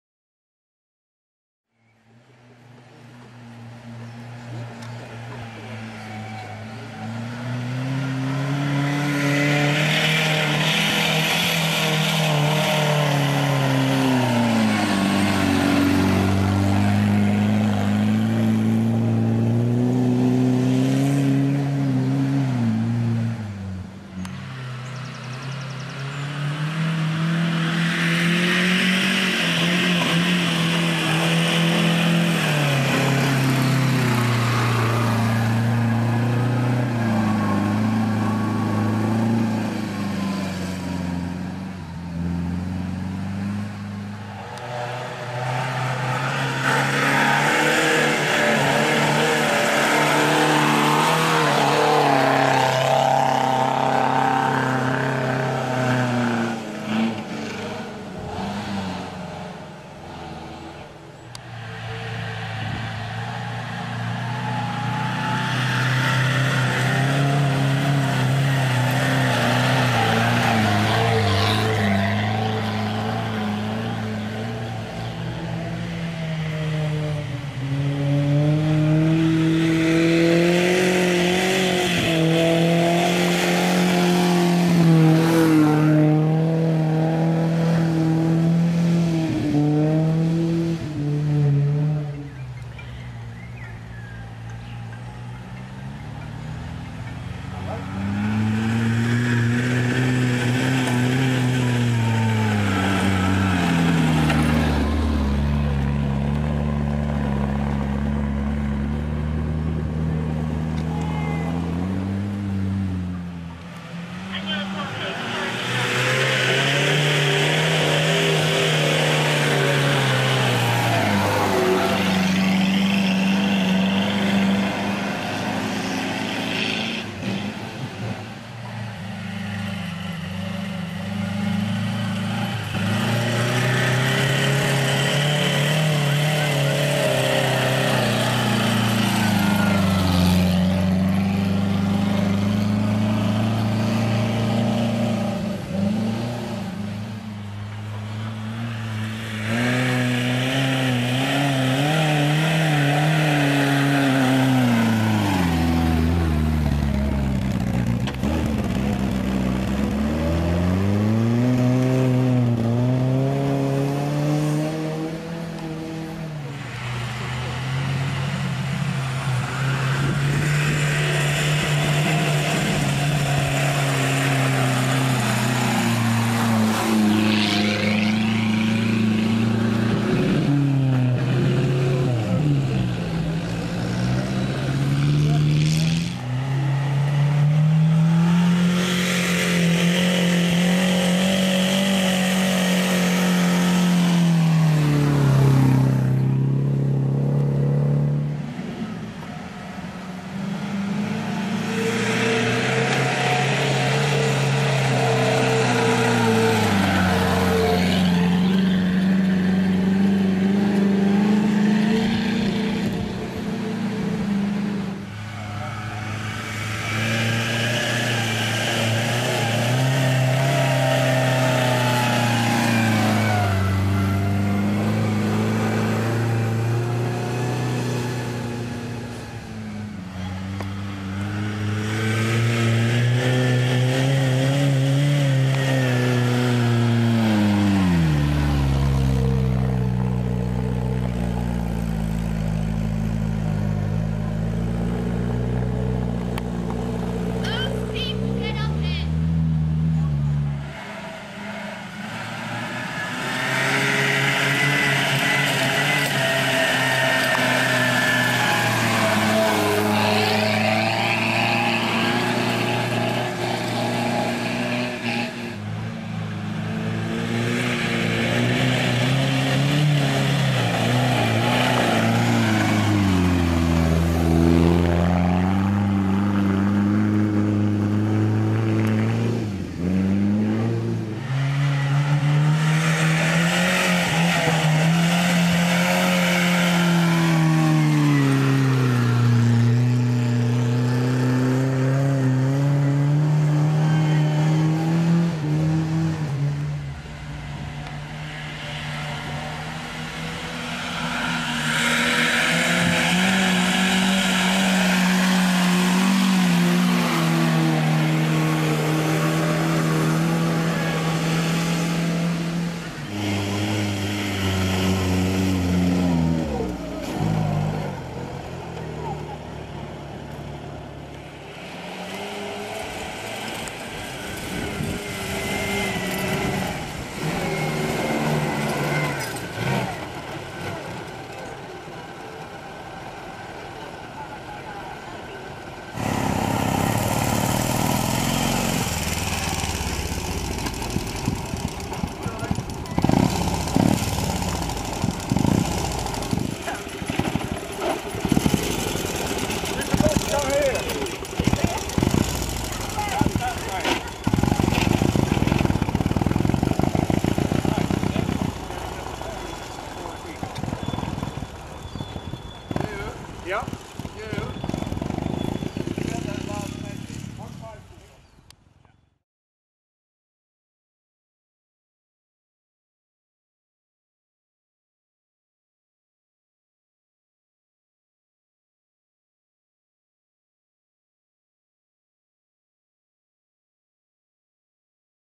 Austin 7s on VSCC Herefordshire Trial 2017
Various variations of the Austin 7 , including the ABC engine "VAT69" special competing on the 2017 Vintage Sports Car Club Herefordshire Trial. March 18th. Filmed by SIMON LEWIS TRANSPORT BOOKS suppliers of collectable motoring and motor sport books, photos and memorabilia.